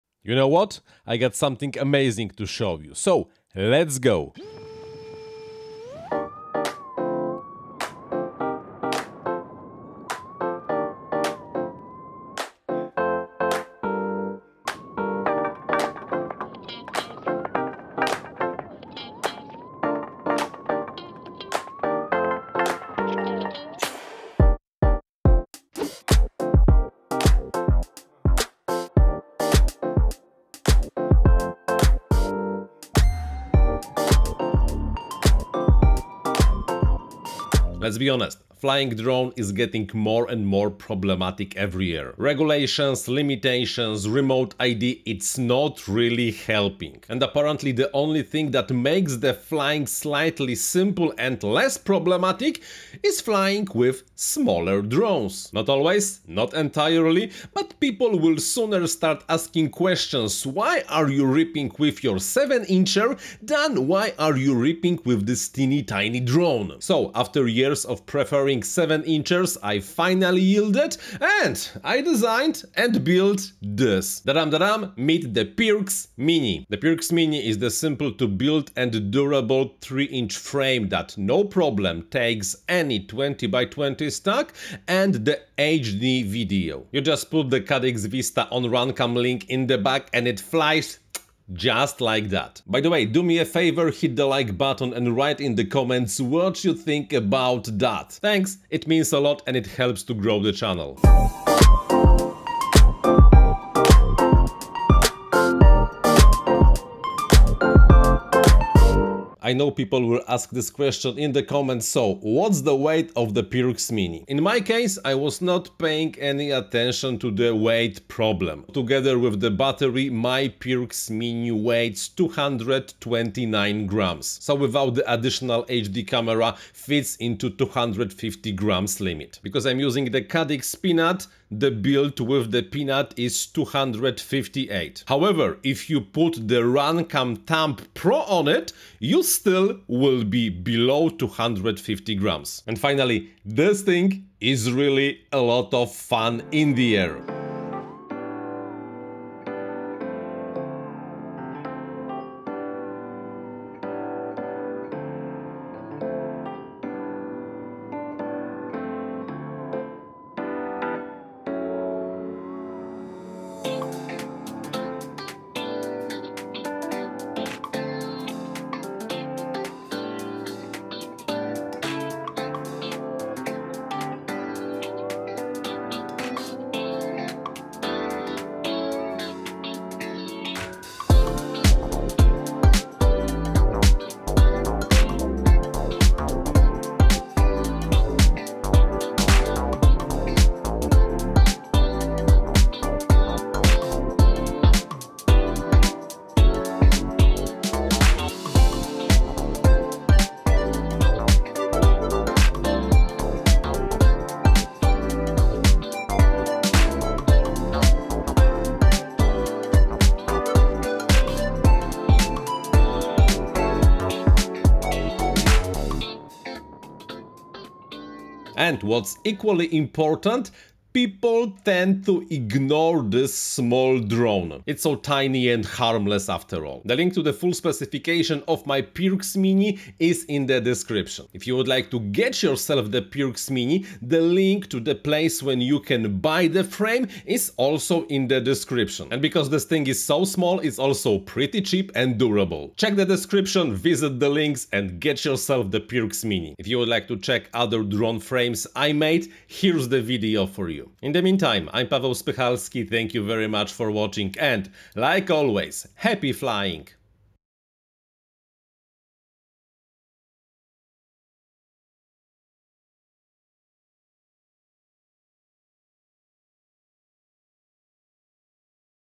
An almost legal, 3-inch FPV drone
Everything is simpler and cheaper with small, sub-250g quads. So I finally  left the "7-inch playground" and made this: tyhe Pirx Mini, no absurds and all-easy 3-inch quad frame. And you know what? I like it!